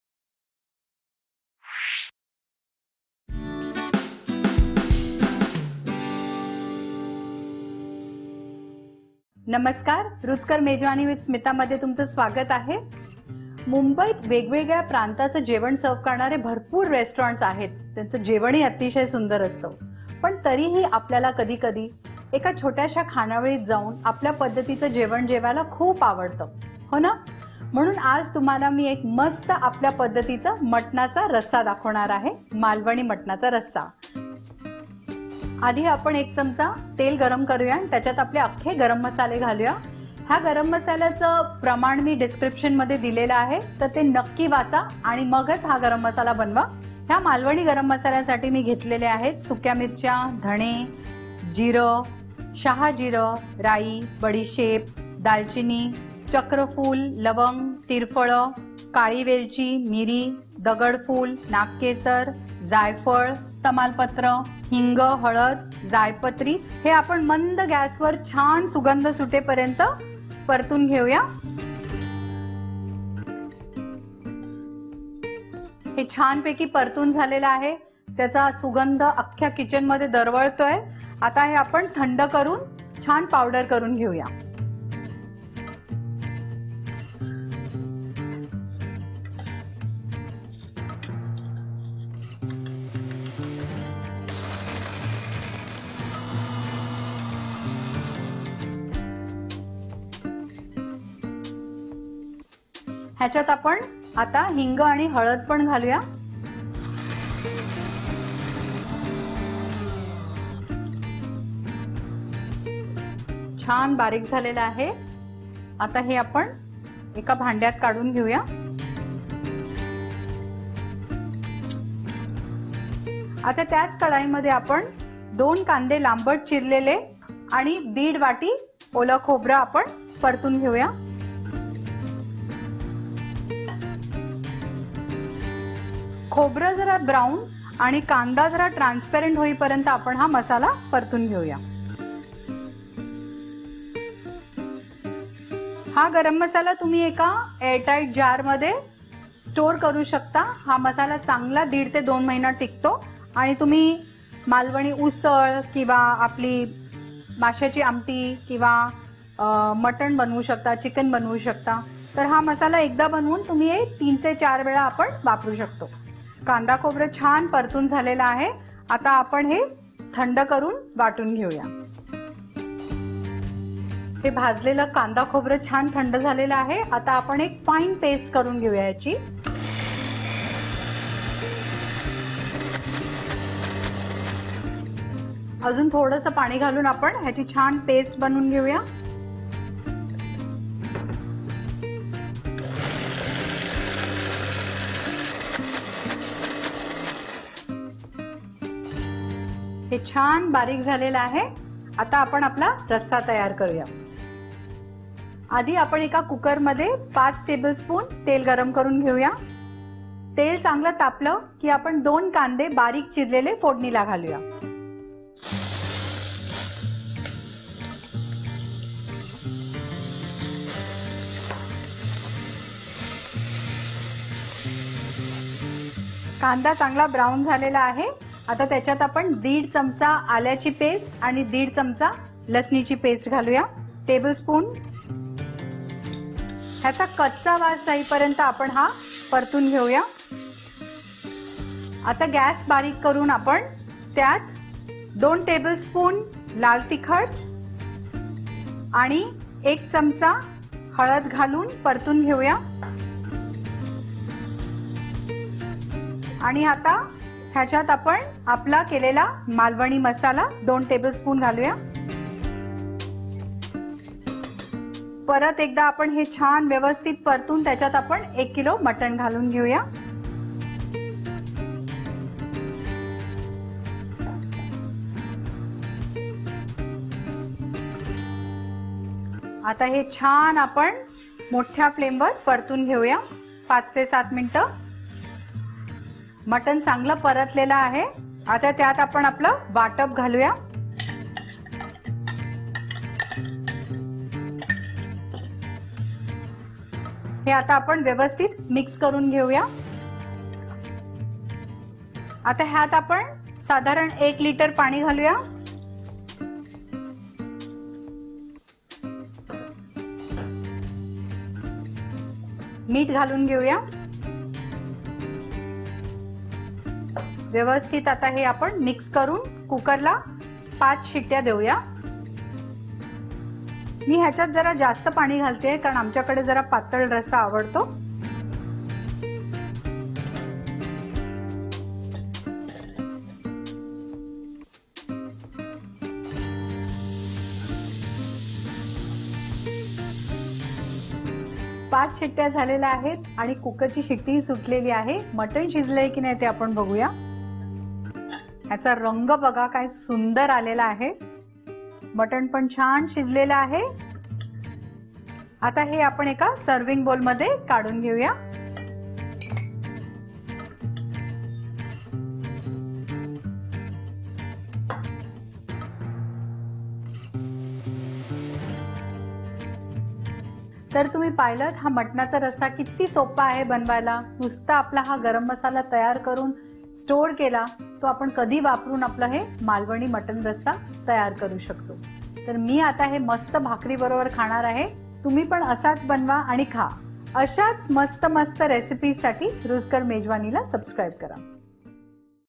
मालवणी मटण रस्सा | Malvani Mutton Curry Recipe | Recipe in Marathi | Smita Deo
Learn How To Make Malvani Mutton Curry Recipe, a Quick and Easy Mutton Curry Recipe in Marathi, मालवणी मटण रस्सा is a Recipe from Chef Smita Deo only on Ruchkar Mejwani. Make this simple and easy Mutton Recipe at your home and share your experience with us in the comments section below.

Ingredients:-
1 kg. Mutton
2 onions, sliced
1½ cups fresh coconut, grated
1½ tbsp. Ginger and garlic paste, each
2 onions, finely chopped
2 tbsp. Malvani masala
2 tbsp. Chilli powder
1 tsp. Turmeric powder
Salt, to taste
5 tbsp. Oil

For Malvani Masala, roast the following spices, cool down to room room temperature and then grind in a  mixer/grinder.
15 red chillies
2 tbsp. Coriander seeds
1 tsp. Cumin seeds
1 tsp. Caraway seeds
½ tsp. Mustard seeds
2 tsp. Aniseeds
1” Cinnamon
1 Star anise
5 Cloves
5 Tirphala
2 Black cardamoms
10 Peppercorns
1 tsp. Stone flower
10 cassia buds
½ tsp. Nutmeg
¼ tsp. Asafetida
1 tsp. Turmeric
1 mace
2 Bay leaves
1 tbsp. Oil

Method:-
1. Roast the coconut and the onions until they change colour and then grind to a fine paste.
2. Heat oil and fry the onions until golden brown. Then add ginger and garlic paste and sauté until the raw smell goes off.
3. Add chilli powder, turmeric powder, malwani masala powder and mix well. Add mutton and fry it for 5-7 minutes and then add the ground onion and coconut paste and mix well.
4. Add a litre of water and salt and pressure cook the mutton for 4-5. whistles and serve.

Malvani Mutton Curry is ready to eat!

HAPPY COOKING!!!

Host: Smita Deo
Copyrights: REPL